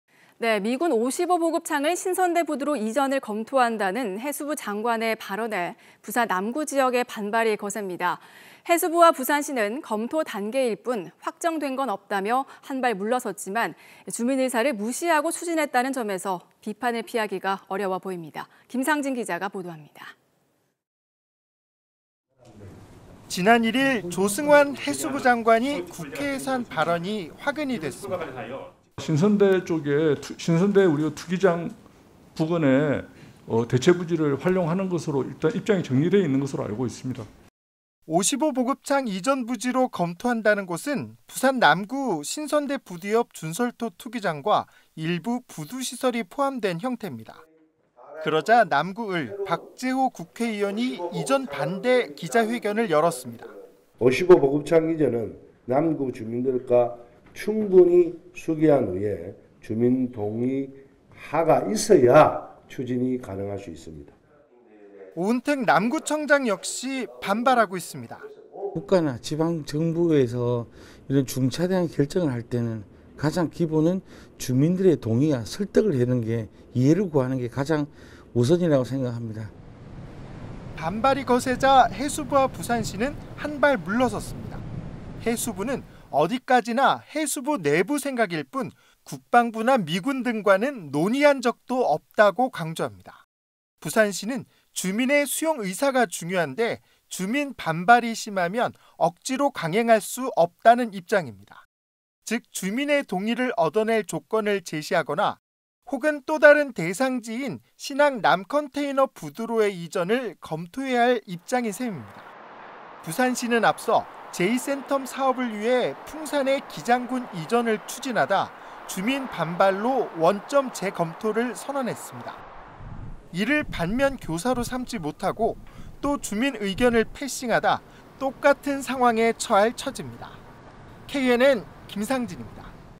55보급창 '결정난 것 없다'...또 다시 표류할까?
[앵커]
미군 55보급창을 신선대부두로 이전을 검토한다는 해수부장관의 발언에 부산 남구지역 반발이 거셉니다.

해수부와 부산시는 검토단계일 뿐 확정된건 없다며 한발 물러섰지만 주민 의사를 무시하고 추진했다는 점에서 비판을 피하기 어려워 보입니다.

김상진 기자가 보도합니다.

[기자]
지난 1일, 조승환 해수부장관이 국회에서 한 발언이 화근이 됐습니다.

{조승환/해양수산부 장관/"신선대 쪽 투기장 부근에 대체부지를 활용하는 것으로 입장이 정리되어 있는 것으로 알고 있습니다"}

55보급창 이전부지로 검토한다는 곳은 부산 남구 신선대부두 옆 준설토 투기장과 일부 부두시설이 포함된 형태입니다.

그러자 남구을 박재호 국회의원이 이전 반대 기자회견을 열었습니다.

{박재호/부산 남구을 국회의원"55 보급창 이전은 남구 주민들과 충분히 숙의한 후에 주민동의가 있어야만 추진이 가능할 수 있습니다"}

오은택 남구청장 역시 반발하고 있습니다.

{오은택/부산 남구청장"국가나 지방정부에서 중차대한 결정을 할때 가장 기본은 주민들의 동의와 설득을 얻는 것, 이해를 구하는게 가장 우선이라고 생각합니다"}

반발이 거세자 해수부와 부산시는 한발 물러섰습니다.

해수부는 어디까지나 해수부 내부 생각일 뿐, 국방부나 미군 등과는 논의한 적도 없다고 강조합니다.

부산시는 주민의 수용의사가 중요한데 주민 반발이 심하면 억지로 강행할 수 없다는 입장입니다.

즉 주민의 동의를 얻어낼 조건을 제시하거나, 혹은 또 다른 대상지인 신항 남컨테이너부두로의 이전을 검토해야할 입장인 셈입니다.

부산시는 앞서 제2센텀사업을 위해 풍산의 기장군 이전을 추진하다 주민 반발로 원점 재검토를 선언했습니다.

이를 반면교사로 삼지못하고 또 주민의견을 패싱하다 똑같은 상황에 처할 처지입니다.

KNN 김상진입니다.